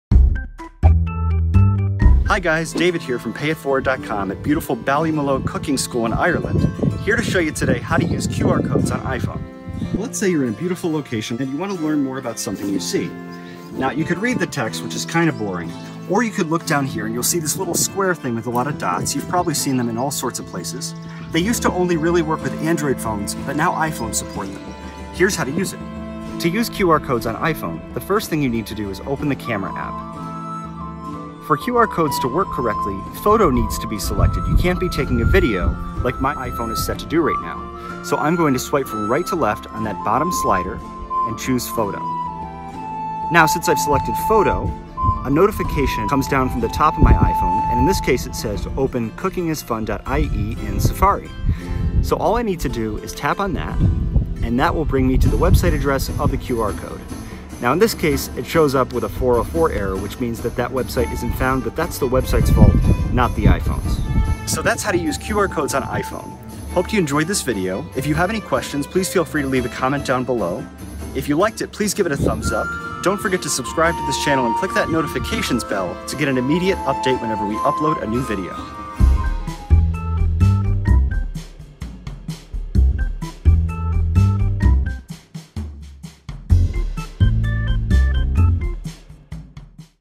How To Scan A QR Code On iPhone
An Apple expert shows you how to scan a QR code on your iPhone! A QR code is one type of matrix bar code you can scan using the Camera on your iPhone. When you scan the code, you receive a notification on your iPhone that typically sends you to a specific webpage.

to learn more!